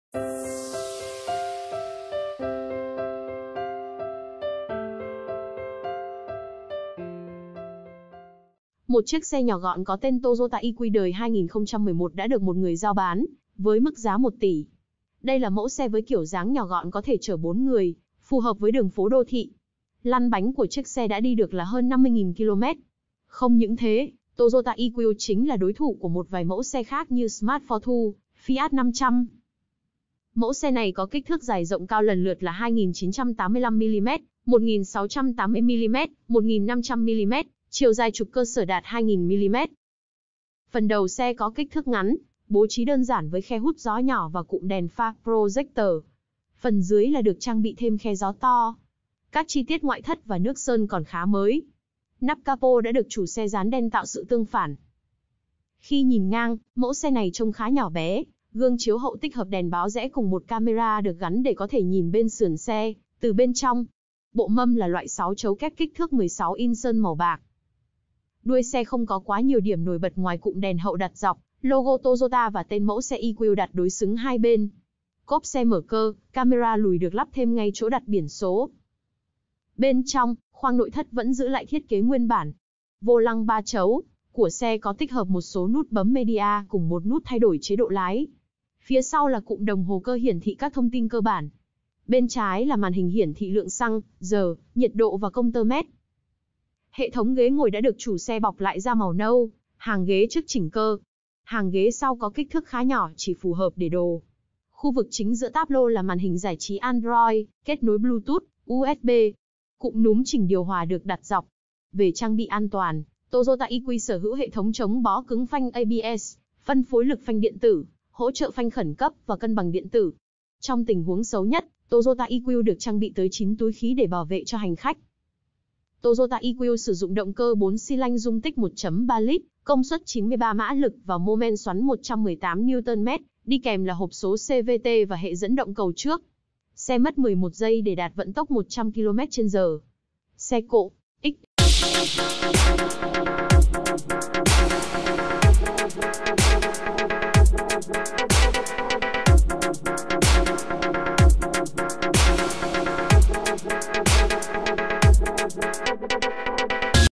"Phát khiếp" với mẫu xe Toyota "siêu có giá" với mức bán khó tin tại Việt Nam khiến ai cũng "nản"
Một chiếc xe nhỏ gọn có tên Toyota iQ đời 2011 đã được một người rao bán với mức giá 1 tỷ đồng. Đây là mẫu xe với kiểu dáng nhỏ gọn có thể chở 4 người, phù hợp với đường phố đô thị. ODO của chiếc xe đã đi được là hơn 50.000 km.